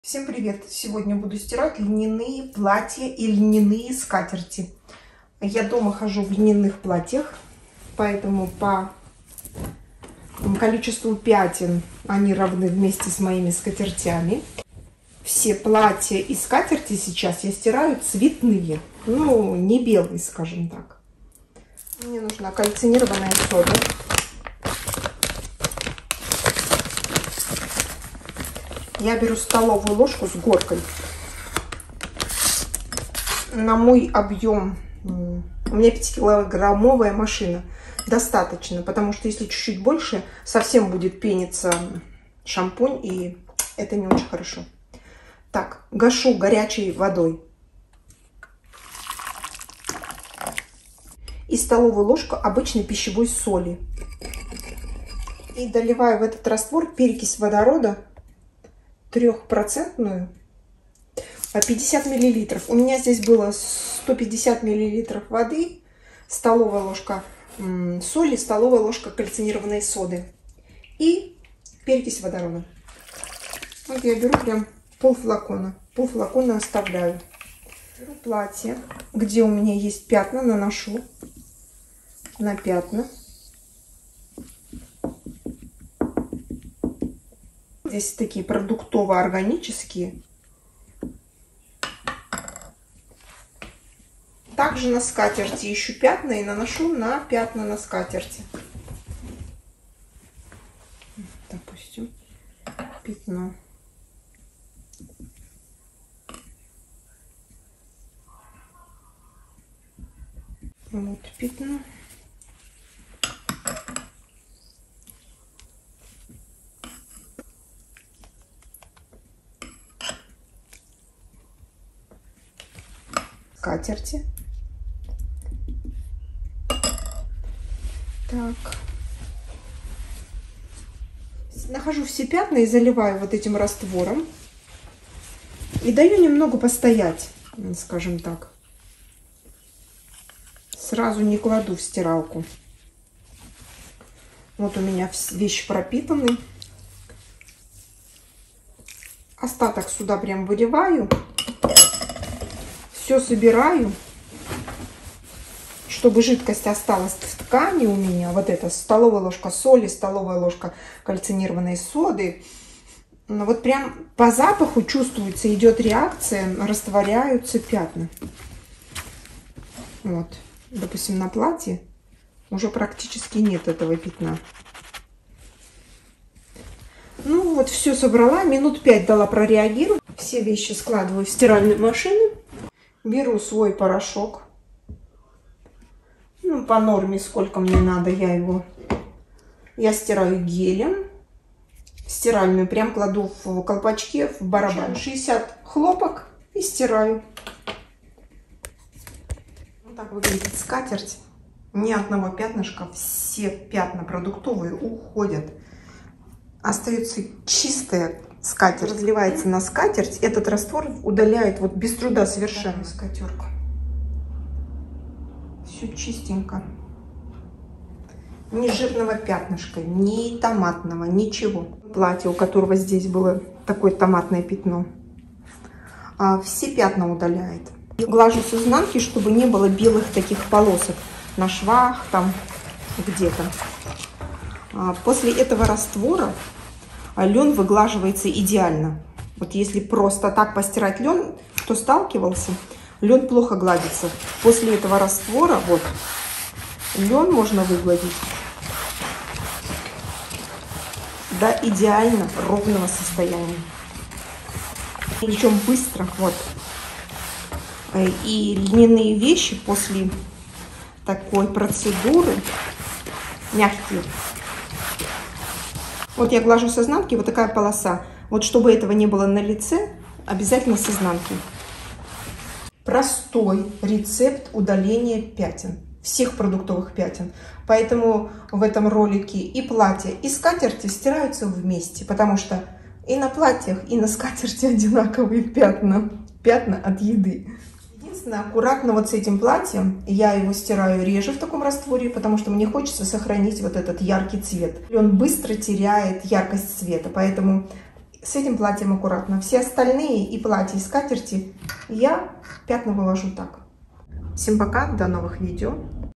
СТИРАТЬ ЛЬНЯНЫЕ СКАТЕРТИ И ПЛАТЬЯ ВЫВЕСТИ ПЯТНА
Льняные платья и скатерти стираю вместе . Пятна вывожу простым способом . И после этого рецепты гладить лен проще .
РЕЦЕПТ :
150 мл горячей воды
1 ст л кальцинированной соды
1 ст л соли
50 мл перекиси водорода 3%